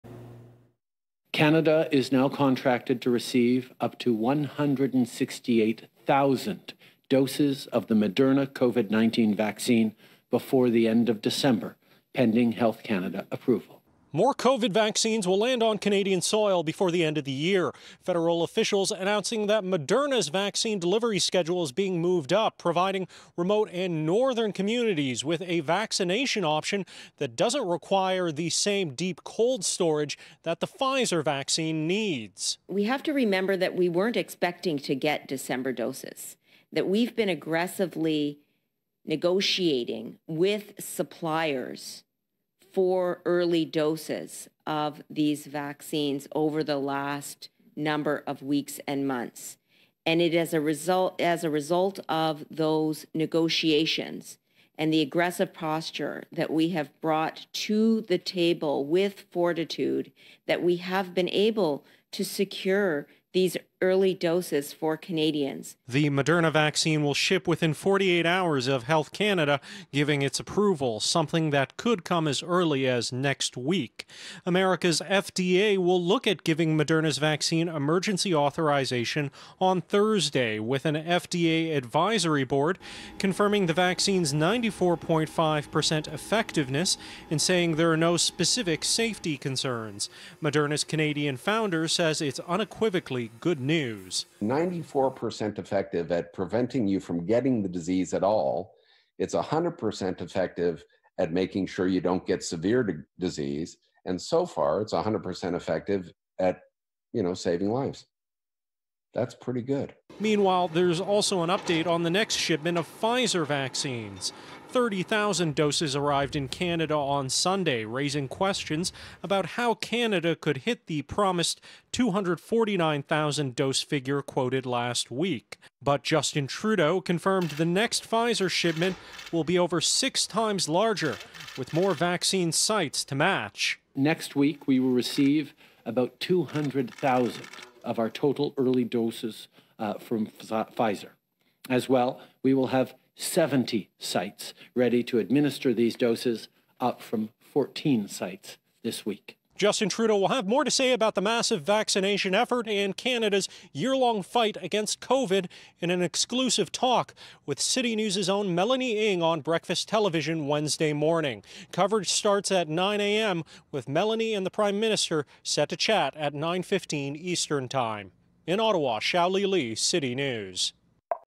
Moderna COVID vaccines to arrive before 2021
168,000 doses of Moderna's COVID vaccine will arrive in Canada before 2021. The vaccines are still yet to be approved, but are ready to ship within 48 hours of Health Canada's green light. Xiaoli Li has more.